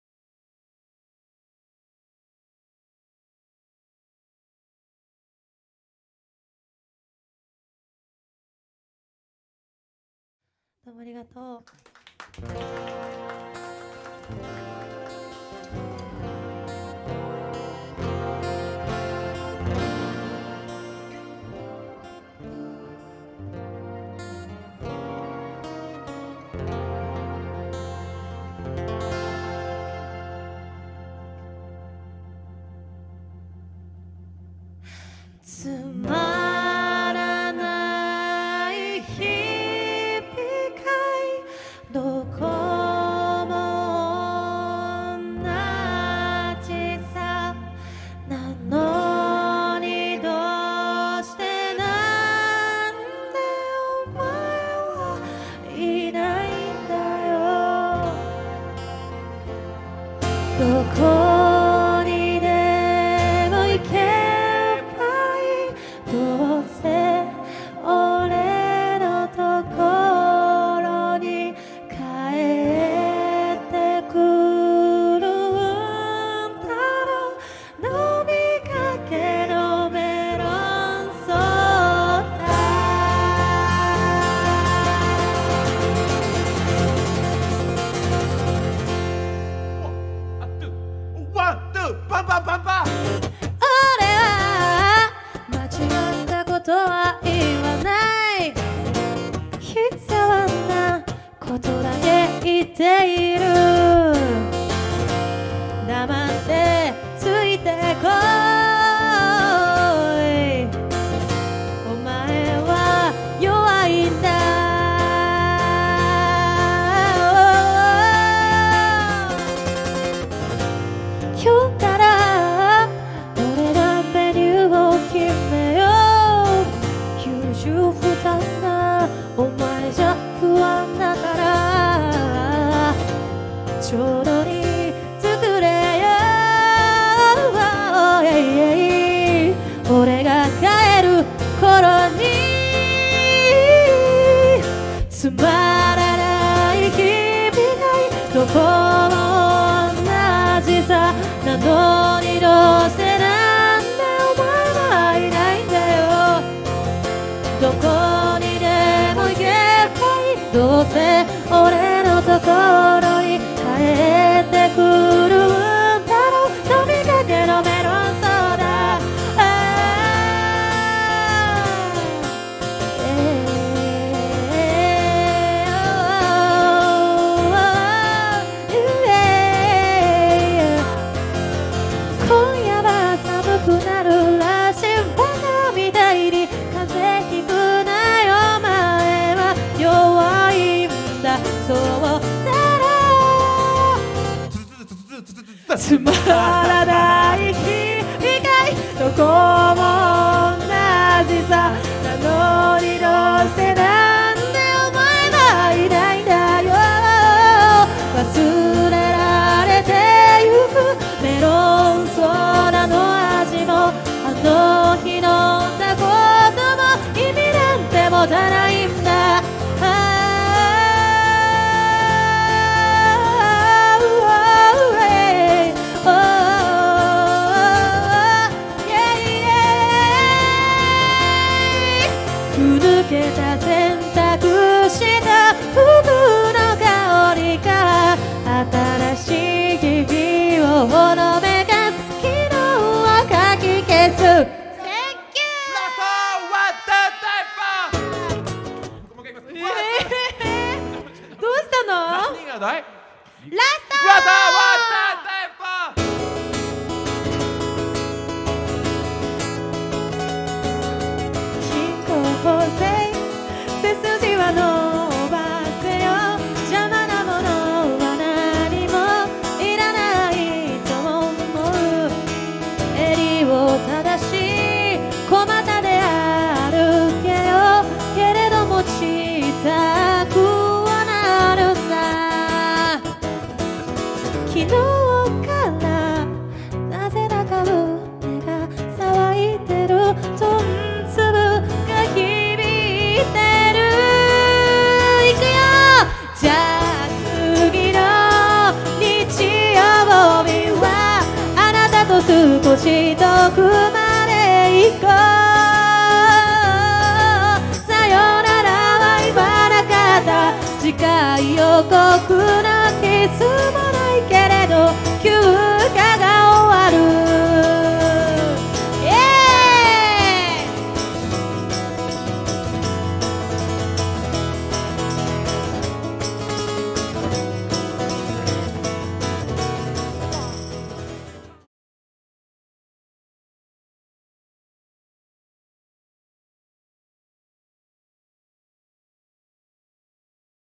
Ikebukuro Adm Online live -Rika&Tomochin(Yonchome No Anna)-
this video is a highlight of a live performance by a artists named "Rika&Tomochin(Yonchome No Anna)" on January 20th, 2021.

1月20日に有観客配信で行われた凛佳＆ともちん（四丁目のアンナ）によるライブ映像ハイライトです。